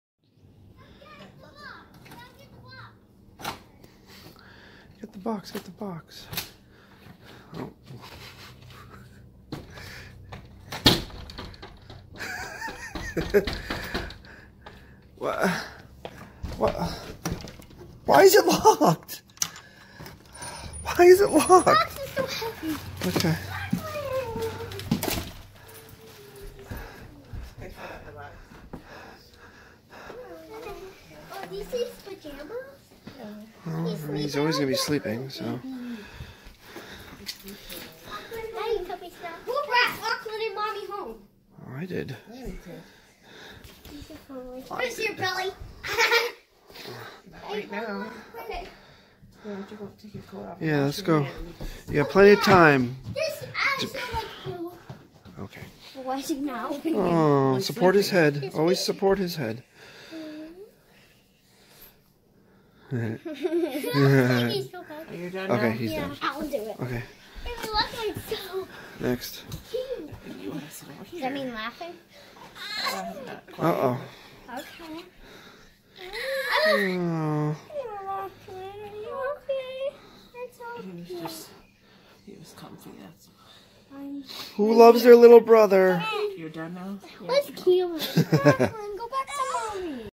1,094 - Lachlan Comes Home (Part 3/3 - Gavin & Ethan)
Gavin & Ethan saw Lachlan in the hospital, but here they are coming home from school seeing him there for the first time. ... BE FRIENDS with JOEL RYAN on FACEBOOK